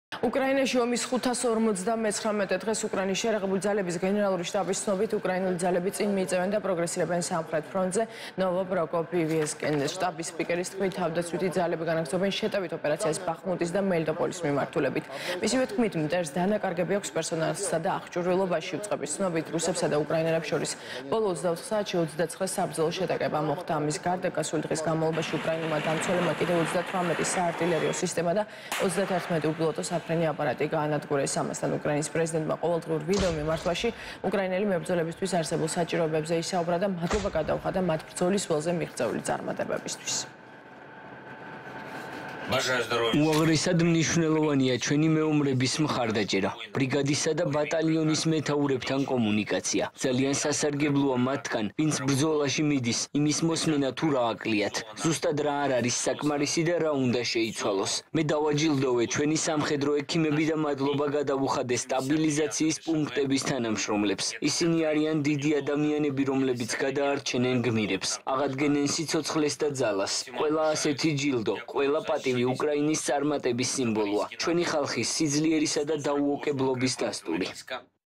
ომის 559-ე დღე - ვითარება უკრაინაში
უკრაინაში ომის 559-ე დღეს, უკრაინის შეიარაღებული ძალების გენერალური შტაბის ცნობით, წინ მიიწევენ და პროგრესირებენ სამხრეთ ფრონტზე ნოვო პროკოპივკასკენ. შტაბის სპიკერის თქმით, თავდაცვითი ძალები განაგრძობენ შეტევითი ოპერაციას ბახმუტისა და მელიტოპოლის მიმართულებით. მისივე თქმით, მტერს დანაკარგები აქვს პერსონალსა და აღჭურვილობაში. უწყების ცნობით, რუსებსა და უკრაინელებს შორის, ბოლო 24 საათში, 29 საბრძოლო შეტაკება მოხდა. ამის გარდა, გასული დღის განმავლობაში უკრაინელმა დამცველებმა კიდევ 38 საარტილერიო სისტემა და 31 უპილოტო საფრენი აპარატი გაანადგურეს. ამასთან, უკრაინის პრეზიდენტმა ყოველდღიურ ვიდეომიმართვაში უკრაინელი მებრძოლებისთვის არსებულ საჭიროებებზე ისაუბრა და მადლობა გადაუხადა მათ ბრძოლის ველზე მიღწეული წარმატებებისთვის.